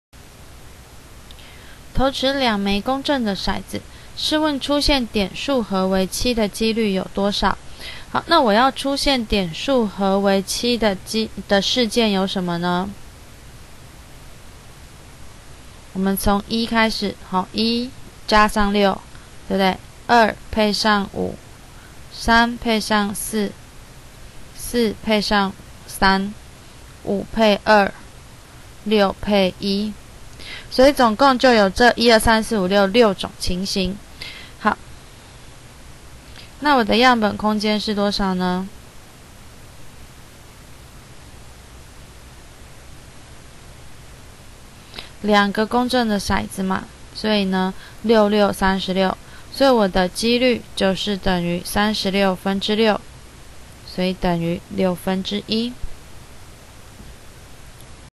技高數學A (四) CH2 自我評量 第10題
由周秉樺老師精心說明，請參考技高數學A版第四冊課本，技高數學A (四) CH2自我評量。